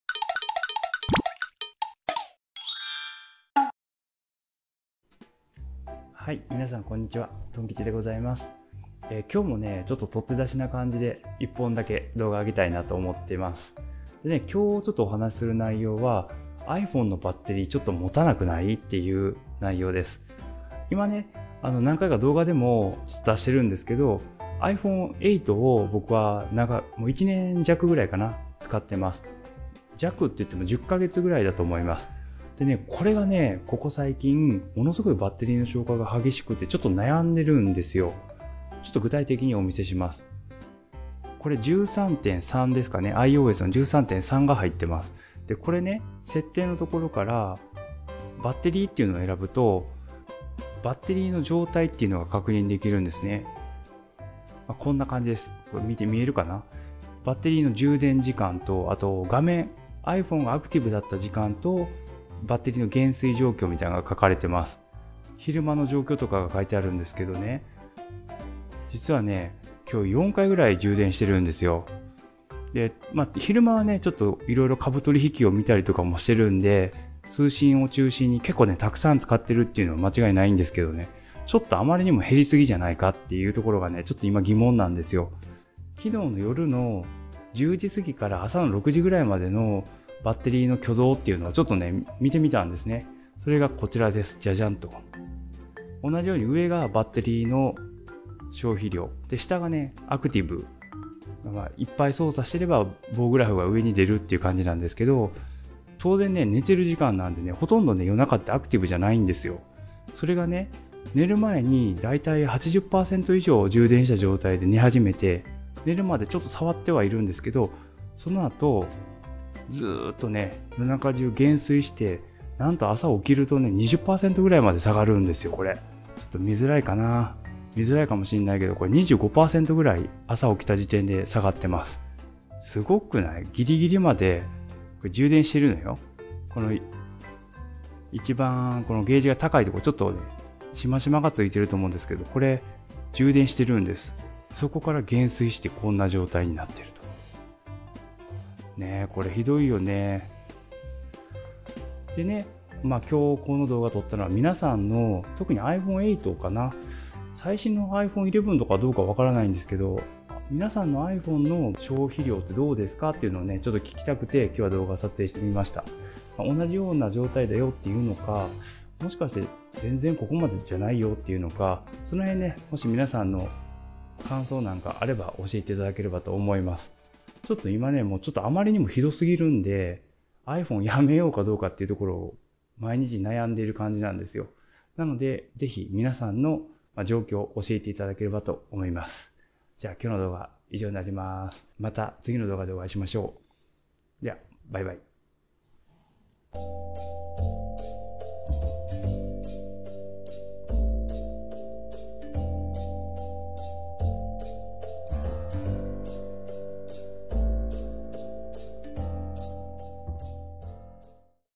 【とって出し】最近iPhoneのバッテリー消化早すぎない（iOS13.3）
最近、iPhone8（使用10か月ほど）のバッテリー消化が早すぎます。
夜間の使っていないときの消化がやばすぎるんです…。
みんなのiPhoneってどんな感じですか？

【撮影機材】
　iPhone8